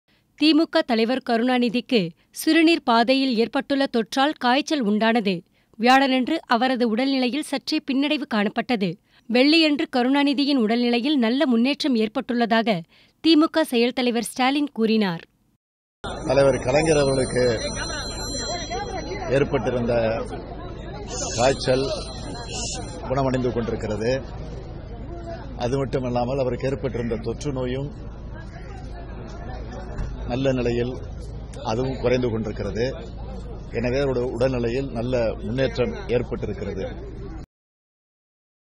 Improvement in Karunanidhi's health : MK Stalin
DMK Leader Karunanidhi's health slightly improved : MK Stalin For more videos